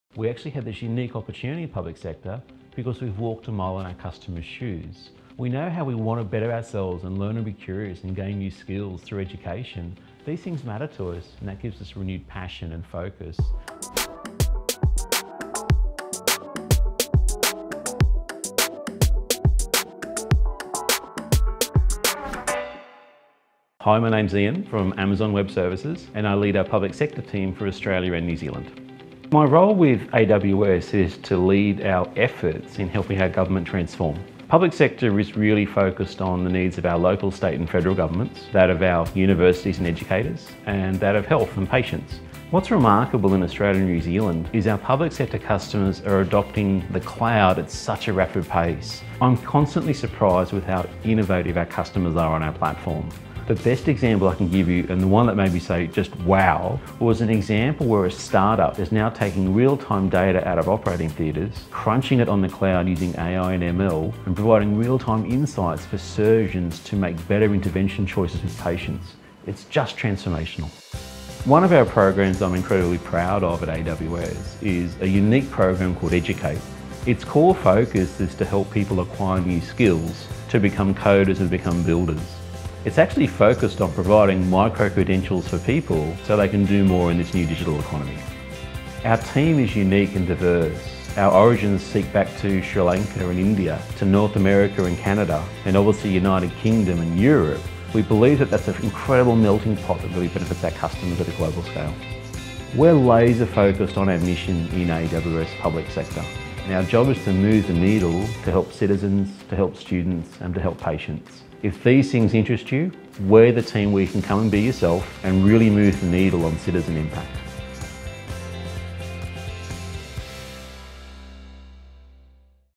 Meet Iain Rouse, Country Manager of Public Sector team at AWS Australia & New Zealand
Our Public Sector team is dedicated to building the future of tech for our communities. Meet Iain Rouse, Country Manager of Public Sector ANZ, as he shares more on what motivates him at work every day.